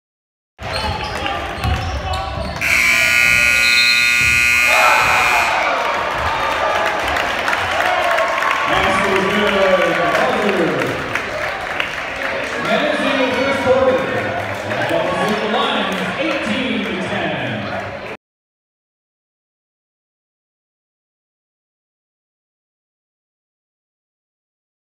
Newaygo’s Wyatt Mortensen hits buzzer-beater from well beyond half-court
The Lions senior banks in a shot to end the first quarter in his team’s 66-31 loss to Holton on Monday, Feb. 22, 2021, in Holton, Mich.